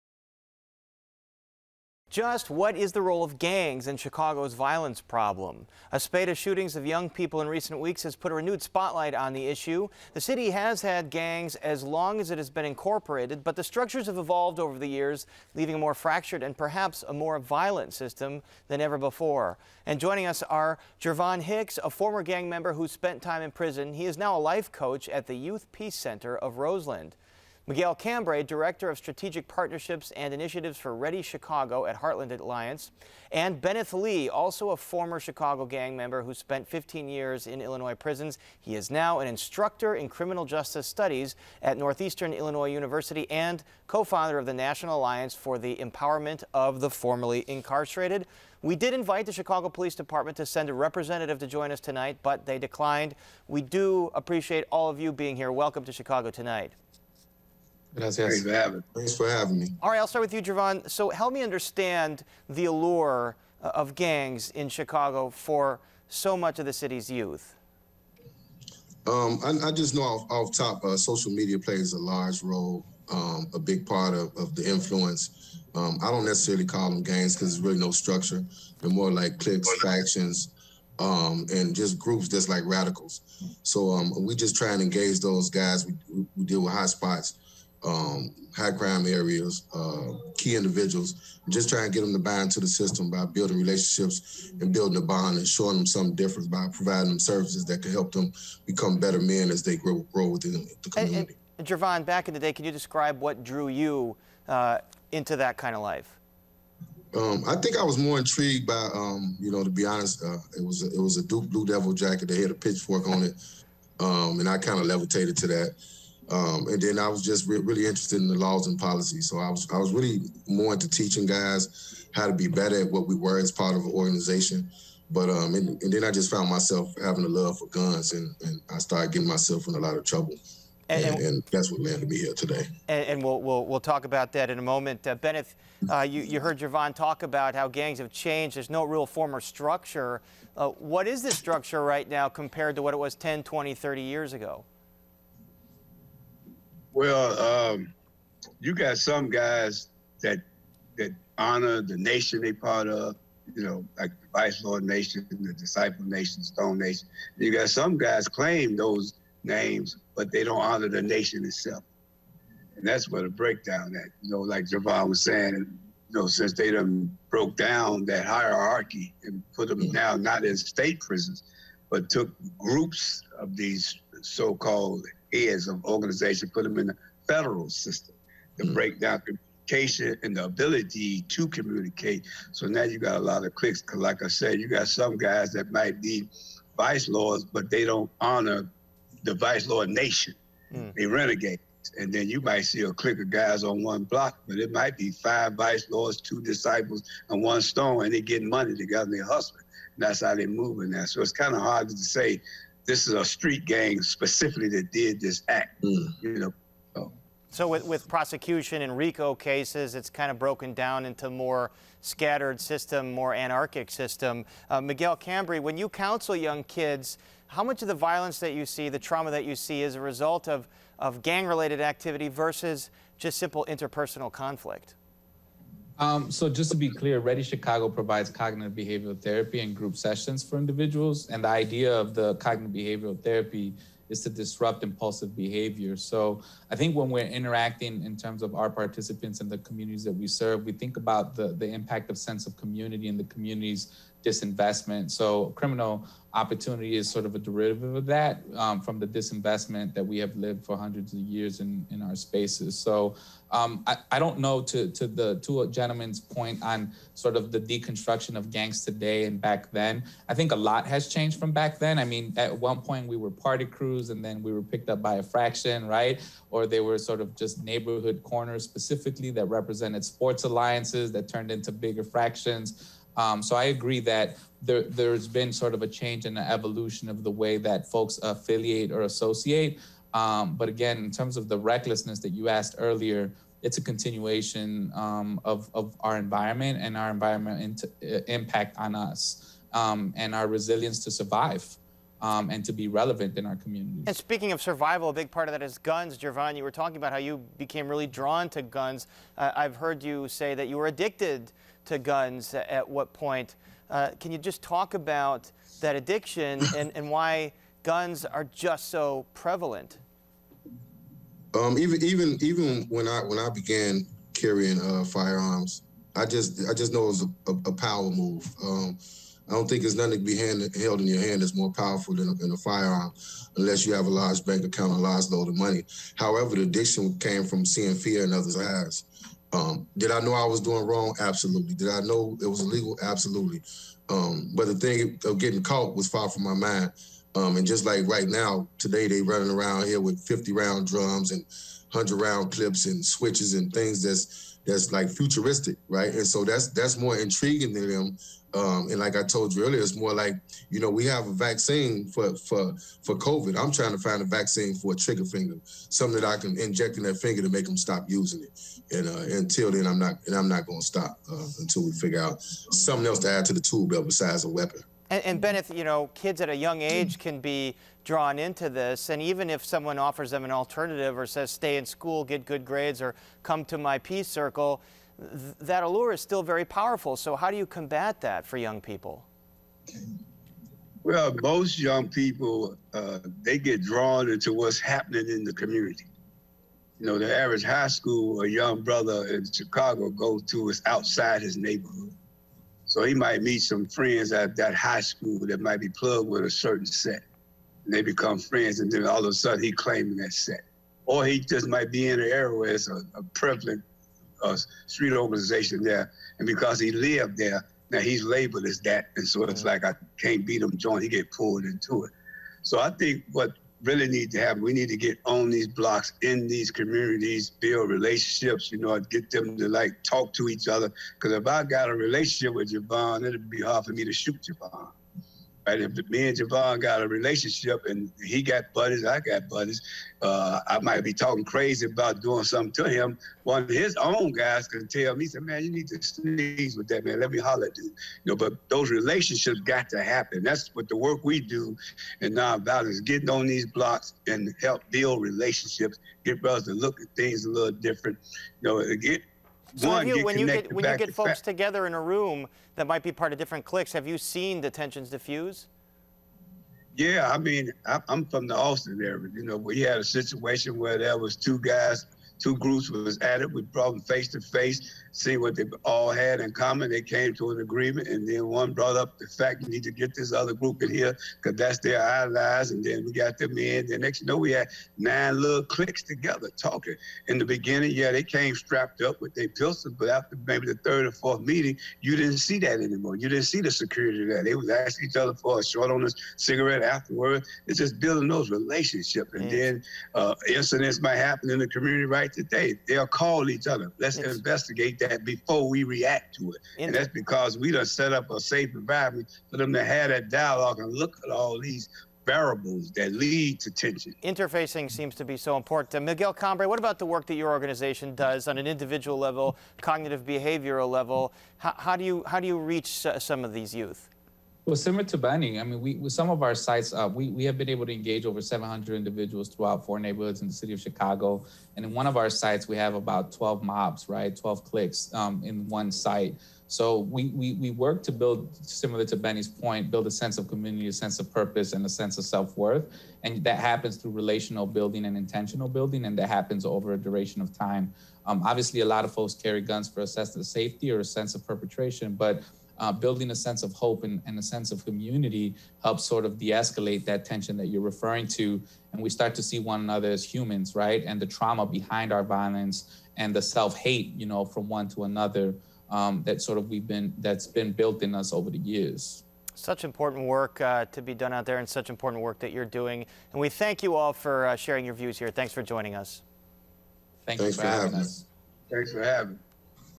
Fractured Chicago Gangs Lead to Anarchic Culture of Violence
The connections between Chicago's gangs and gun violence and what can be done to help young people access other options.